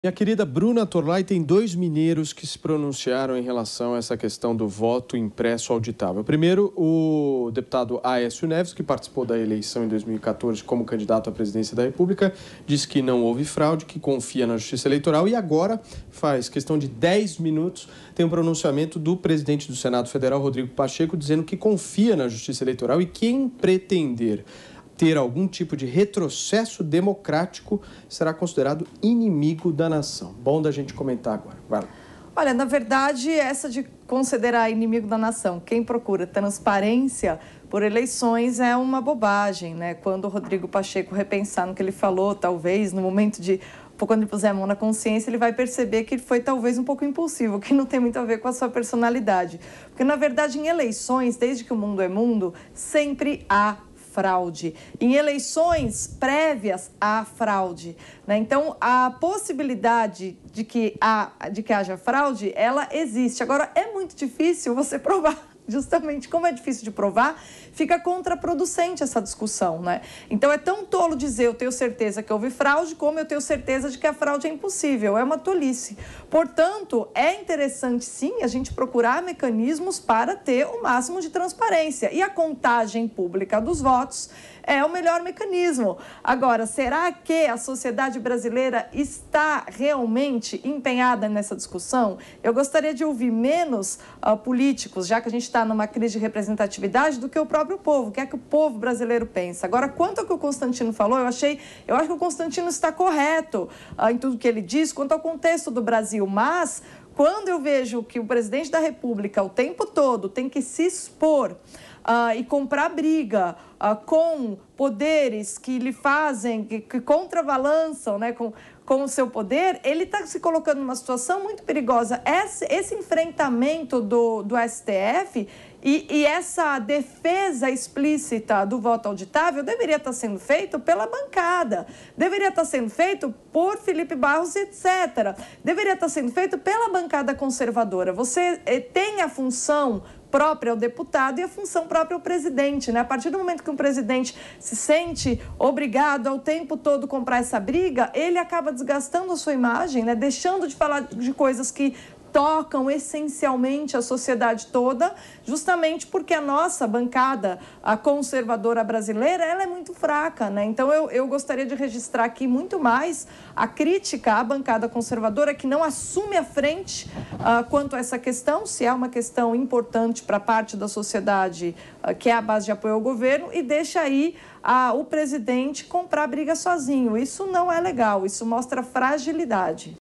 Bruna Torlay: É importante termos mecanismos para promover o máximo de transparência dos votos
Bruna Torlay analisa como a existência da possibilidade de fraudes nas eleições pede pela procura de mecanismos de controle mais eficazes, como a contagem pública de votos, e critica a falta de postura por parte da bancada conservadora